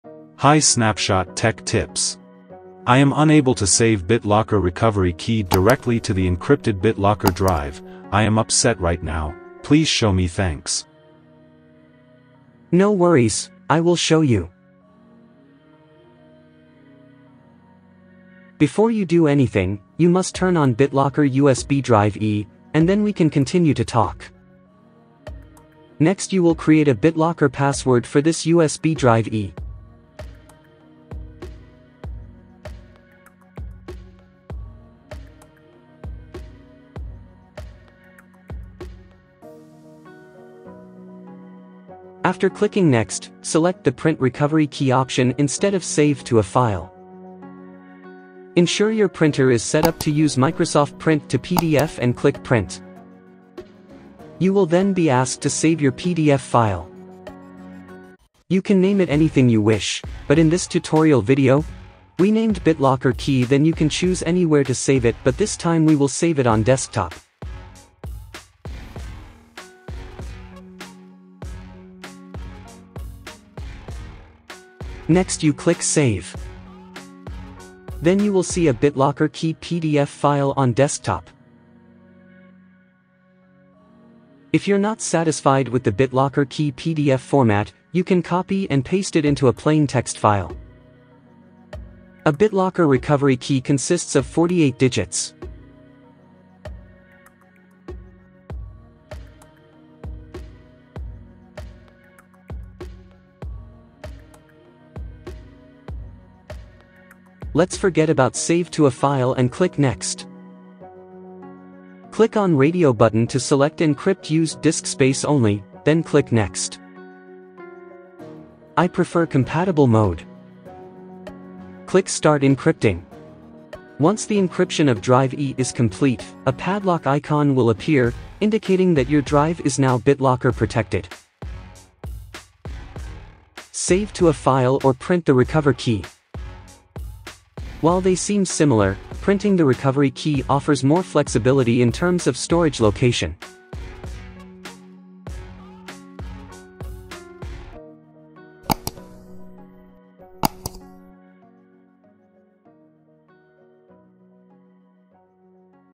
How Can You Save a BitLocker Recovery Key on an Encrypted Drive?
Imagine your BitLocker Recovery Key as the key to a vault filled with your most precious digital possessions. If this key falls into the wrong hands, your data could be compromised. Storing it on an encrypted drive adds an extra layer of protection, ensuring that even if someone gains access to your device, they still won't be able to unlock your encrypted data. However you should do a second backup into the Microsoft account, it is a free of charge and third layer of backup is that you do reverse your BitLocker key and save it a safe place.
By combining encrypted storage, a Microsoft account backup, and saving it on a piece of paper. You're now creating a robust security strategy that significantly reduces the risk of data loss.
Hint
Any critical data should always be backed up to a second physical hard drive in case it is lost or damaged with time.